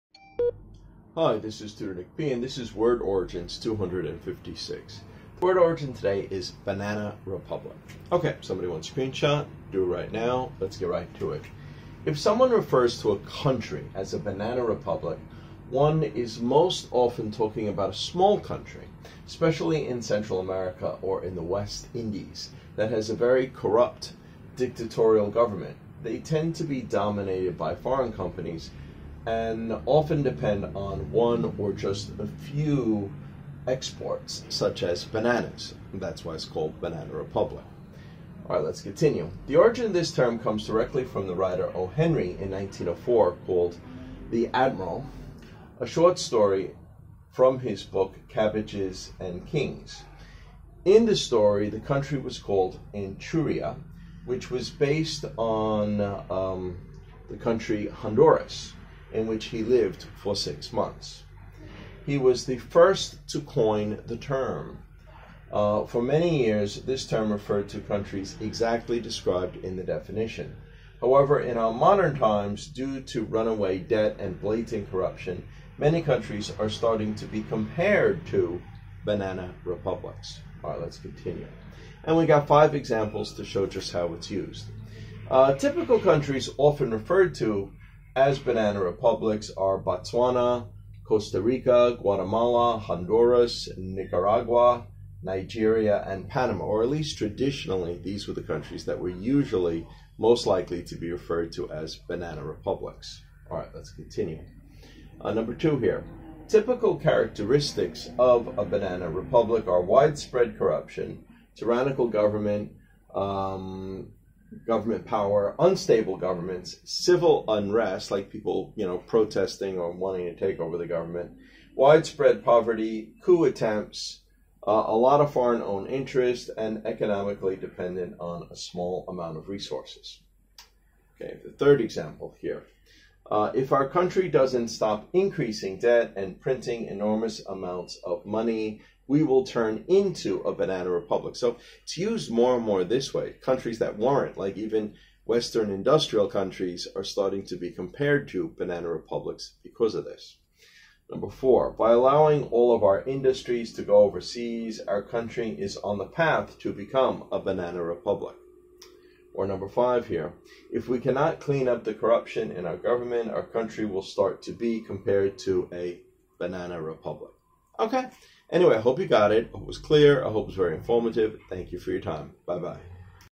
English Tutor Nick P Word Origins (256) Banana Republic
In this video, we will cover the meaning of this phrase , give its origin and give examples of its use.